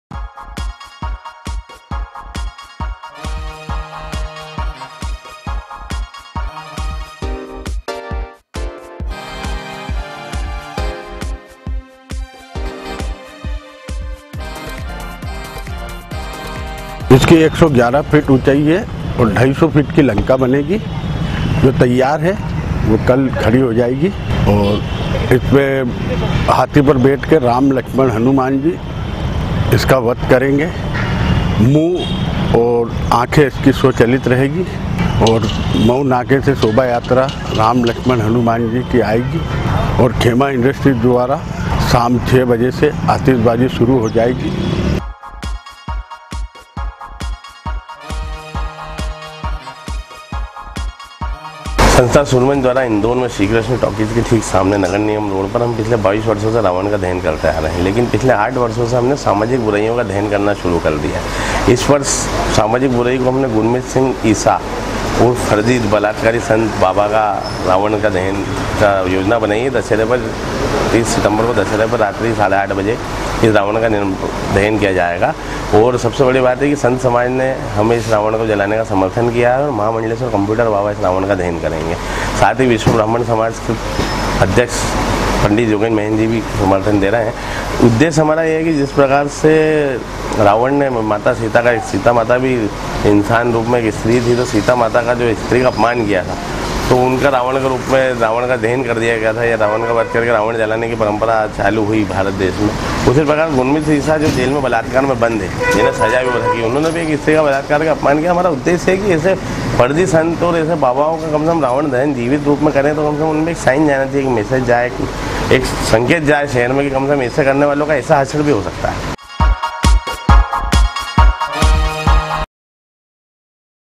राम रहीम, दशहरे पर दहन
इंदौर में दशहरे पर जलेंगे भांति-भांति के रावण